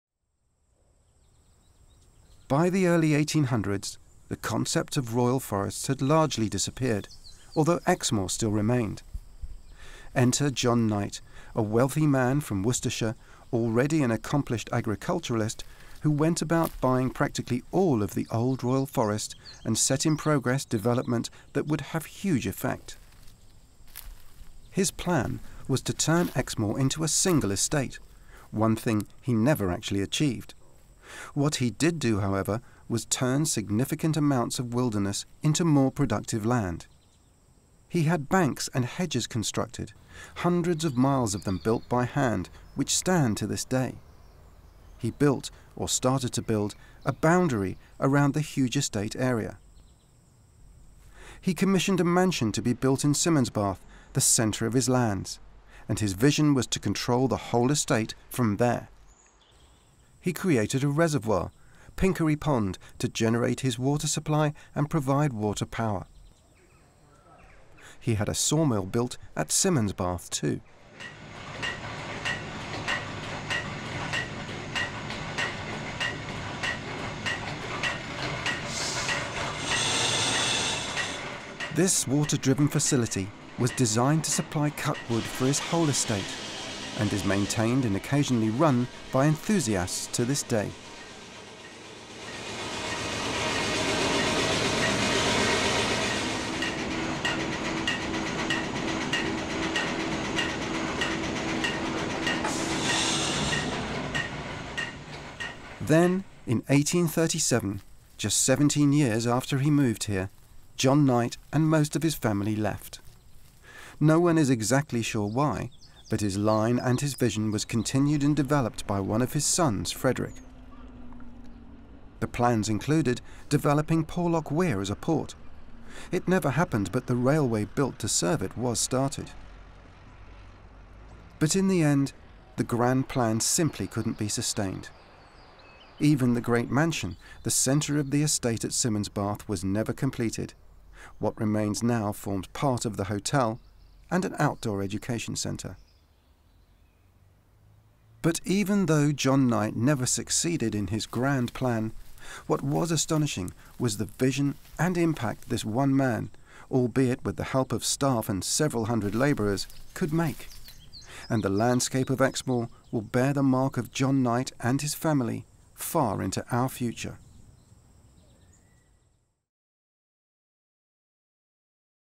11 A KNIGHTS TALE
How did one man's vision leave an impact of the Exmoor landscape that is still clearly seen today?